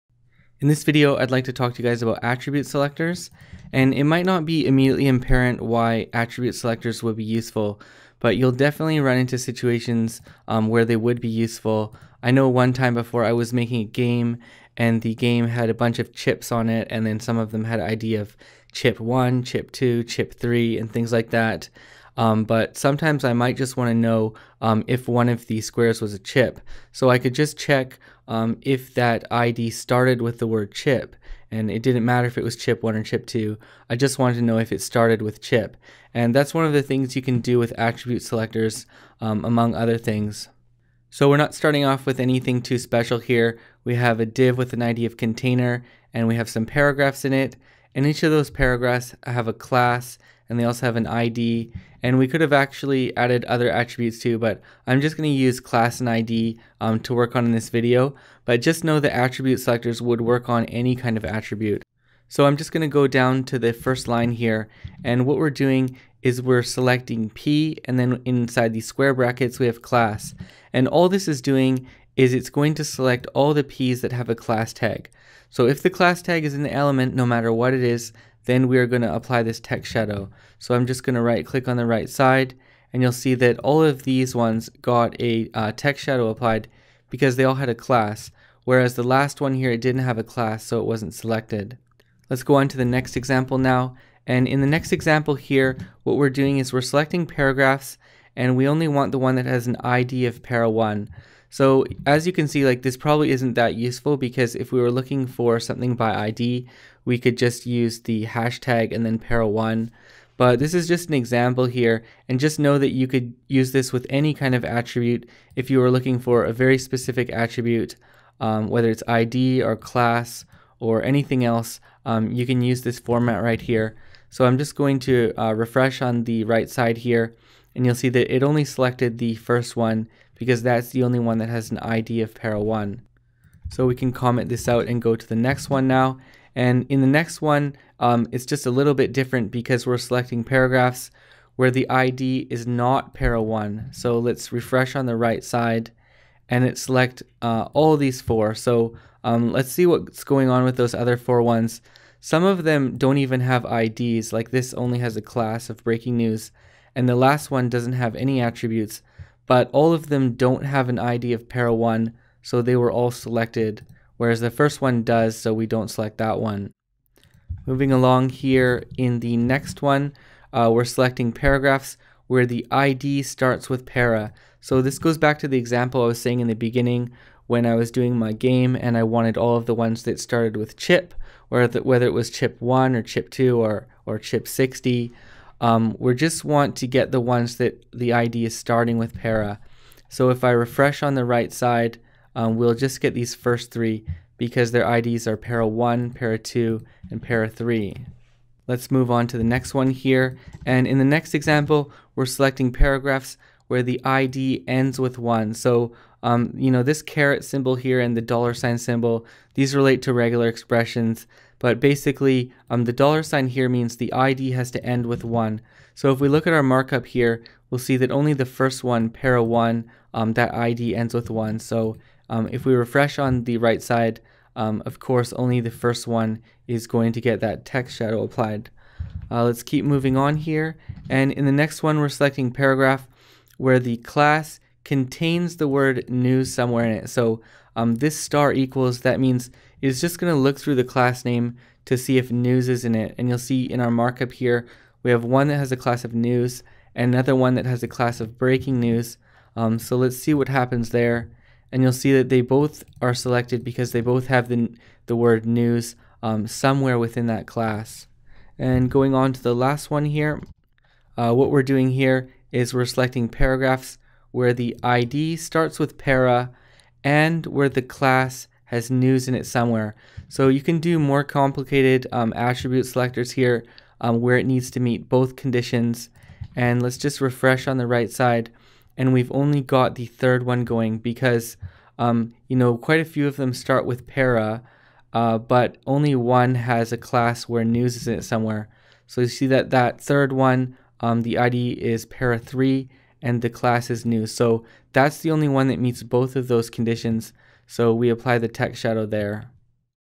Beginner jQuery Tutorial 9 - Attribute Selectors
Selecting elements based on attributes. Eg. What an attribute starts with, what an attribute ends with, what it contains, and other factors.